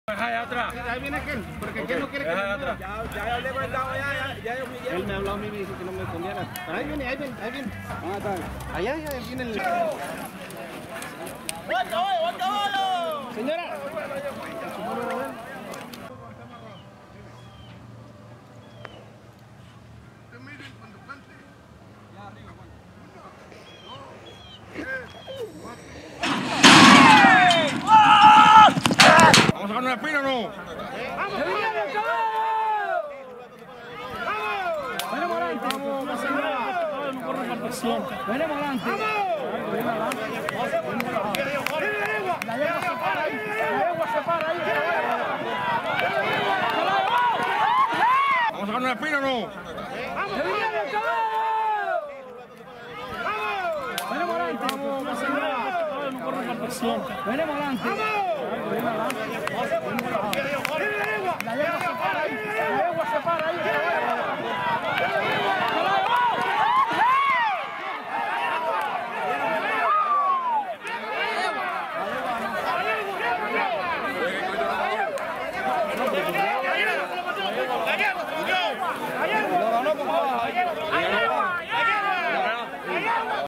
La Esperanzita vs El Comando
Rancho El Venado 30 de Enero 2022
La Esperanzita Cuadra MVP Jockey Miguel
vs
El Comando Cuadra El Venado Jockey Alex Carrillo